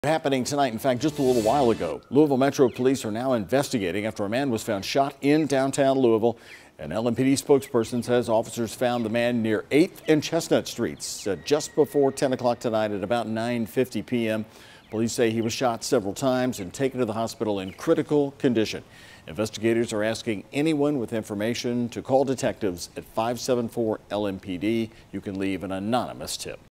Man shot near downtown Louisville, LMPD says
Police said that victim was found in the 800 block of West Chestnut Street and was taken to the hospital in critical condition Tuesday night.